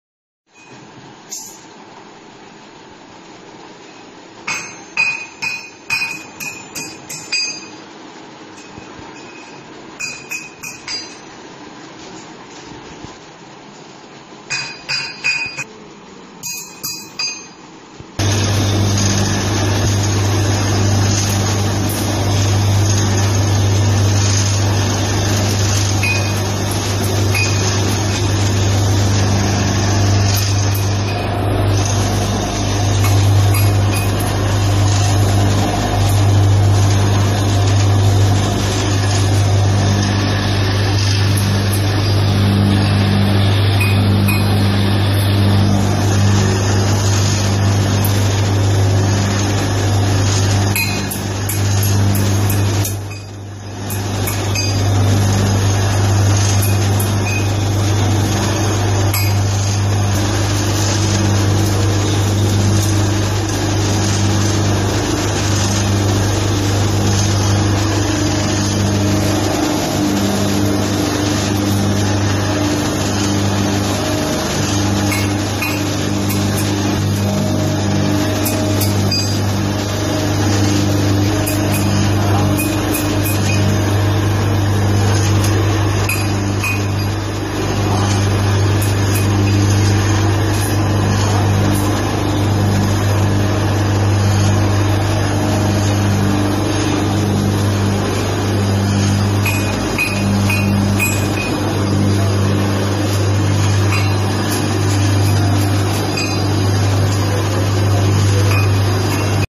Slitting /fabric roll cutting machine
Machine can cut all type of roll shape material like fabric -non woven fabric- plastic tapes  fome tape length 70, inches O D 15,inches
advance apparel technology @yahoo.com